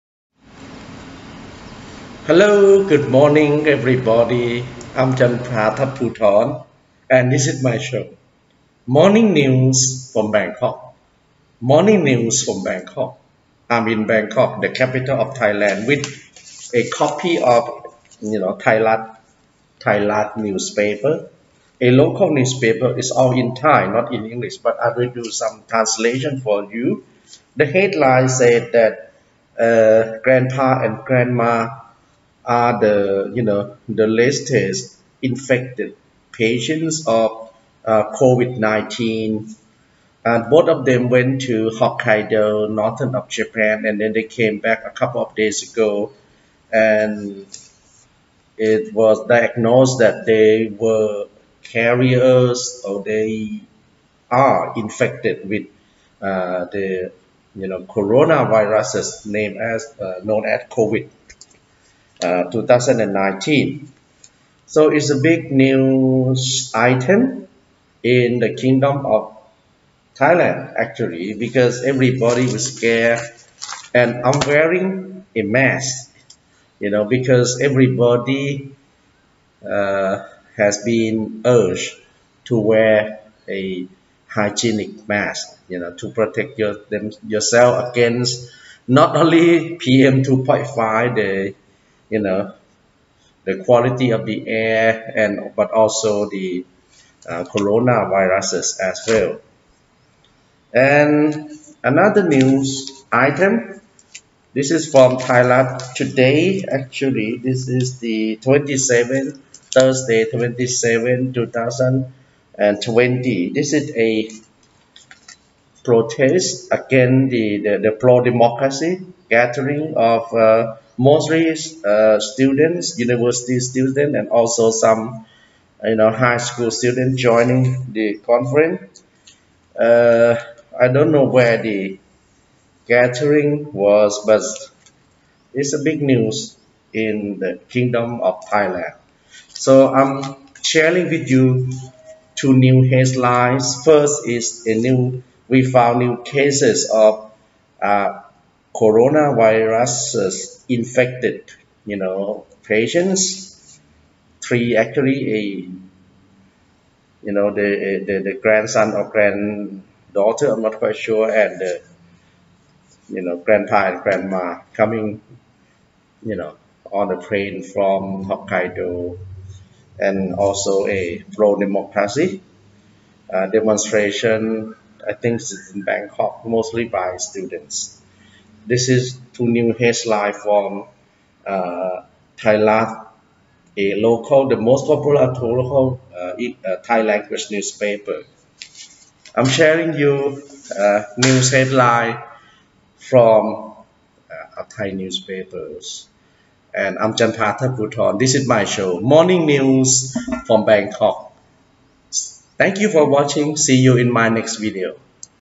Morning News from Bangkok- News Headlines from Thairath, a local newspaper on Feb 27th, 2020
Two news headlines from Thairath
1. New cases of Corona Viruses infected Thais
2. Pro-democracy demonstration